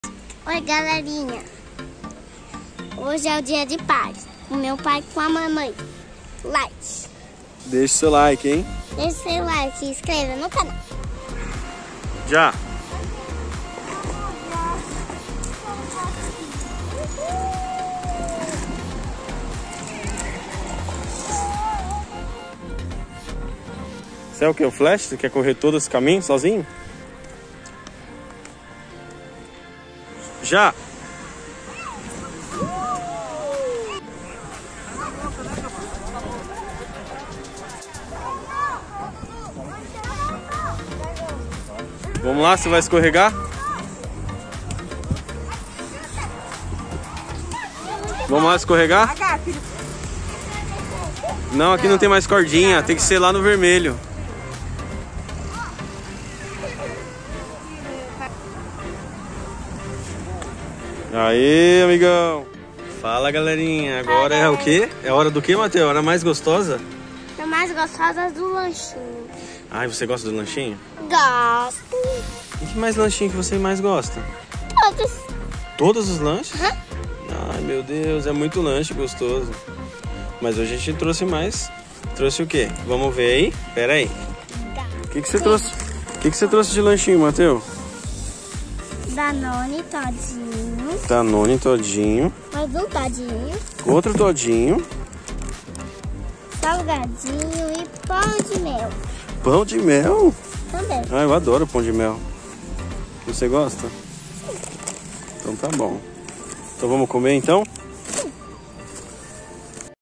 Brincando no Parque
Fala Galerinha Topizera abençoada! Mais um vídeo saindo do Forno. Nesse vídeo gravamos um passeio breve no parque. (Seguimos todos os protocólos de segurança).

Foi muito divertido. Estamos com vários projetos e em breve gravaremos. 😍😍😁.

Sigam nossa Página no Instagram @robs.kids.games

Ótima semana a todos.